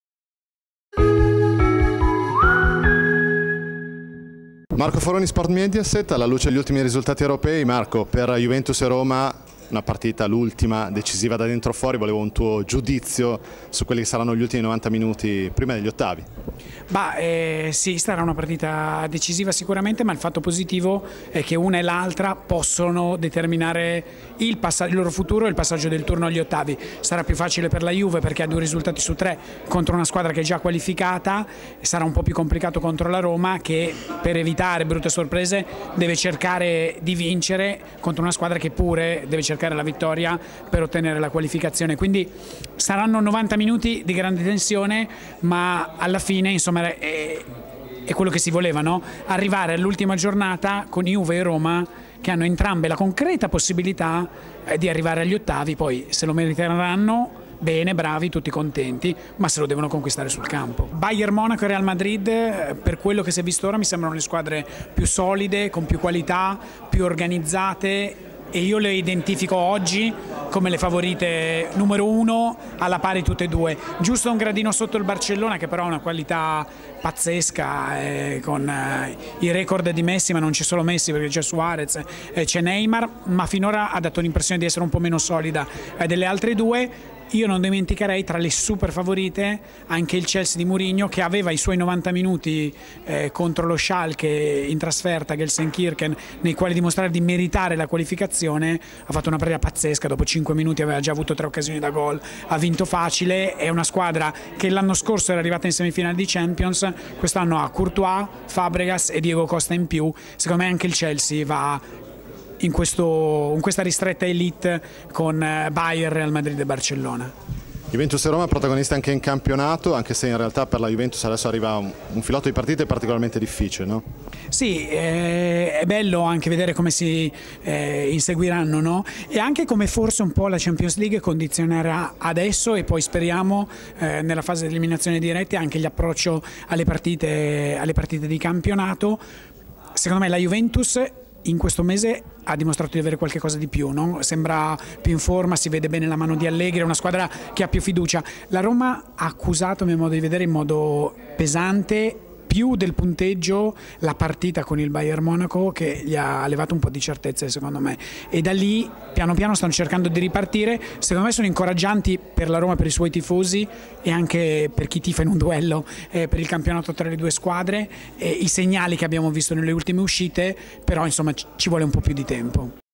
Marco Foroni: "In Champions Juve e Roma attese da novanta minuti di grande tensione!"
Il popolare giornalista di Sport Mediaset, dopo il pareggio della Roma e la vittoria della Juventus, analizza il cammino delle italiane in Champions League, a pochi giorni dall'ultima decisiva sfida dei rispettivi gironi.